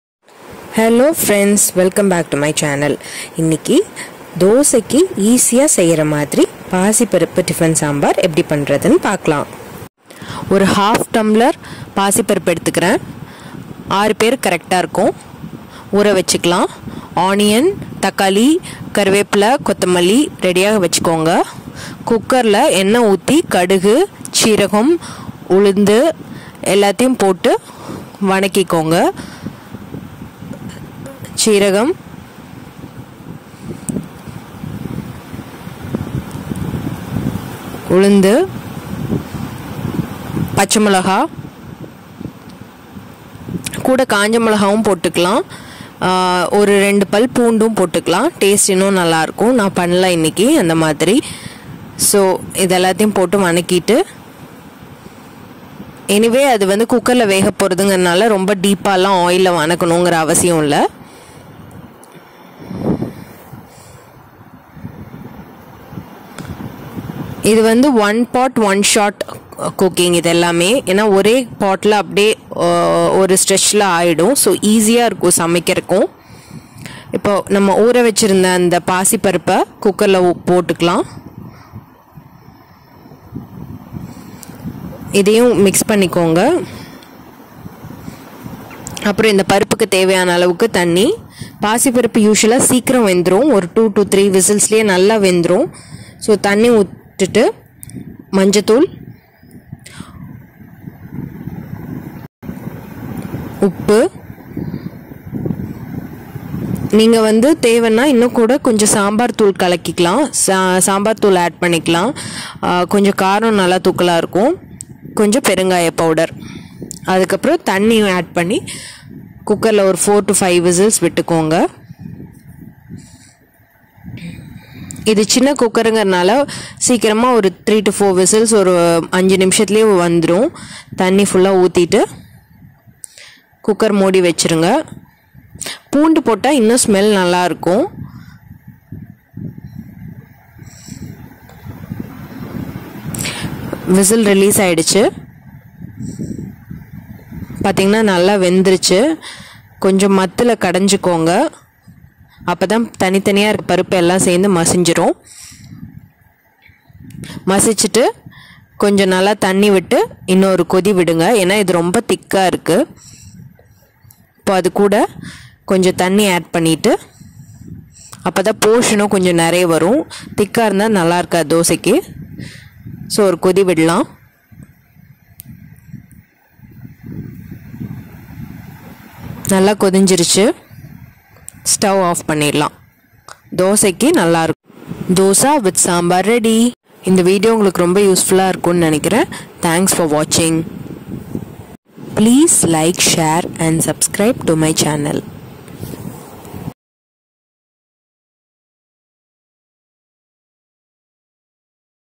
Pasiparuppu Tiffin Sambar
Hello Friends,
This is an  One pot one shot recipe.(OPOS).

Bored of regular coconut chutney, try this Easy Tiffin sambar.
This is not only yummy, but also a best way to keep your portion control. You can have this cup of sambar for just one dosa and control your carbohydrate intake.
For those who have never tried this, try and let me know your comments.
Thanks,
Meena